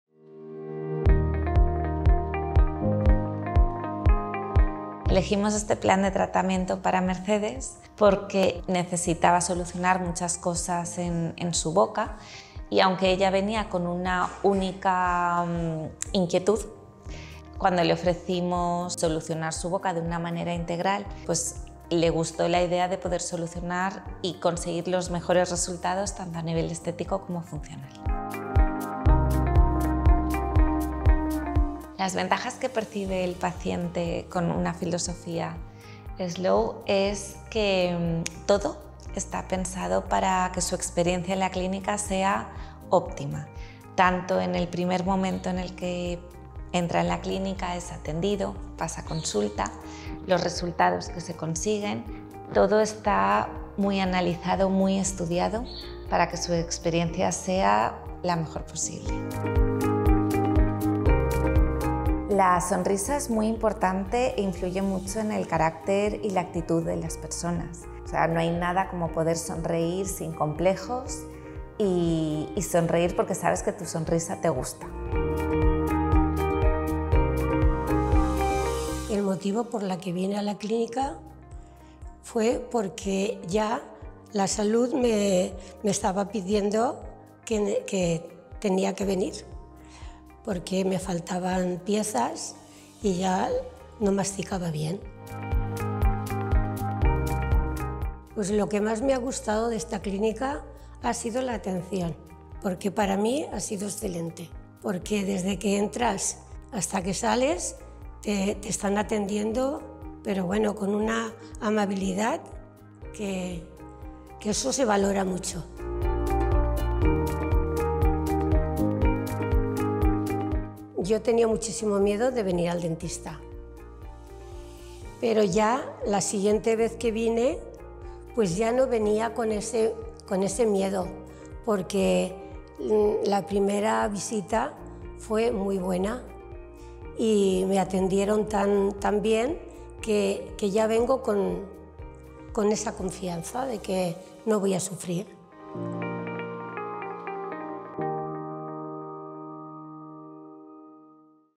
La sonrisa de Mercedes | Clínica W Paloma Valero
“Fui al dentista por salud, me faltaban piezas dentales y no podía masticar bien. Lo que más me ha gustado de Clínica Dental Paloma Valero es la atención porque para mí ha sido excelente. Desde que entras hasta que sales te atienden con amabilidad y eso, se valora mucho”.

Te contamos el caso de Mercedes, paciente de la Clínica Dental Paloma Valero. Tu dentista para toda la vida en Valencia.

¡Síguenos!